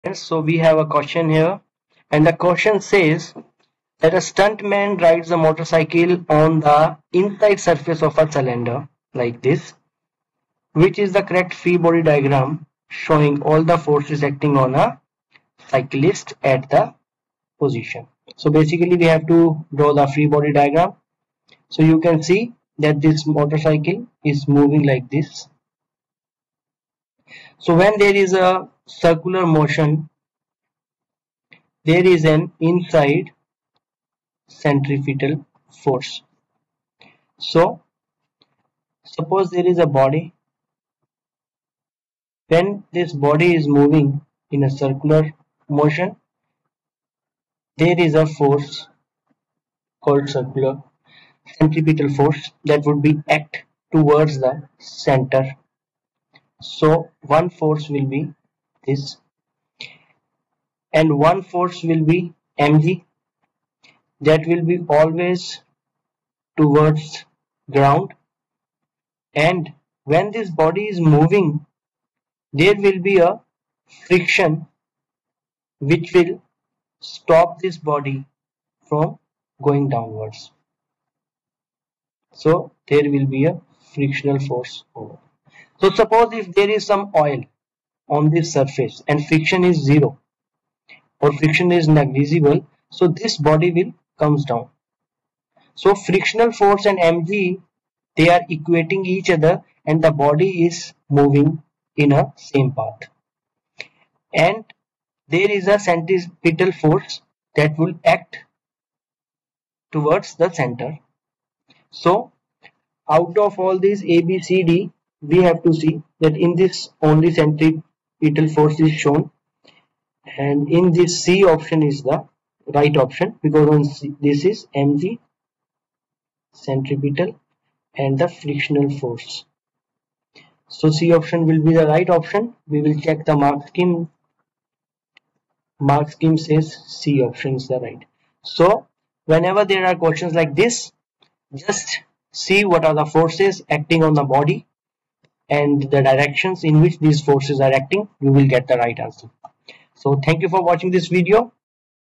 A stuntman rides a motorcycle on the inside surface of a cylinder. Which is the correct free-body
A stuntman rides a motorcycle on the inside surface of a cylinder.
Which is the correct free-body diagram showing all the forces acting
on the cyclist at that position?